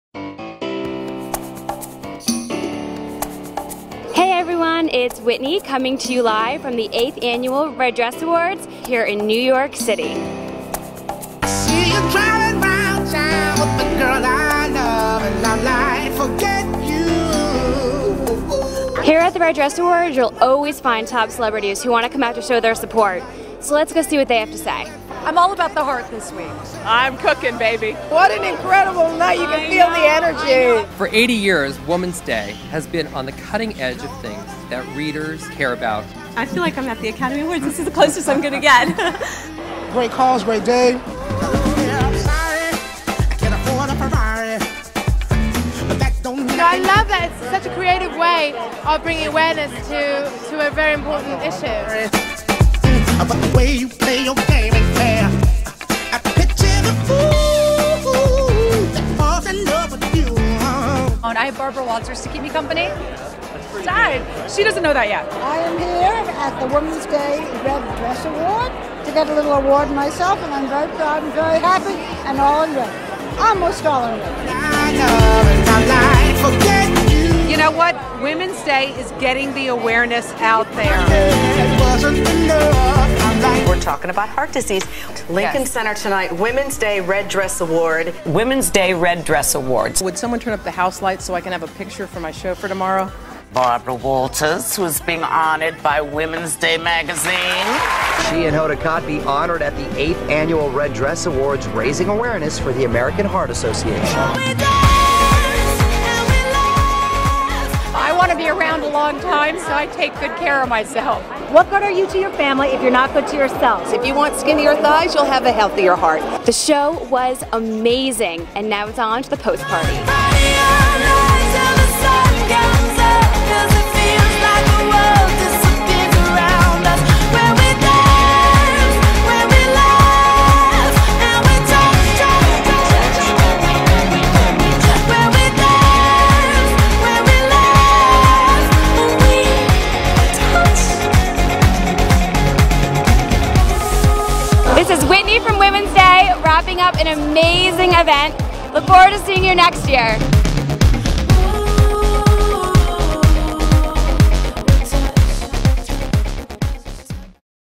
Red Dress Awards (Heart Health) - Woman's Day Magazine
Watch the highlights from Woman's Day's red carpet event to promote heart health in women.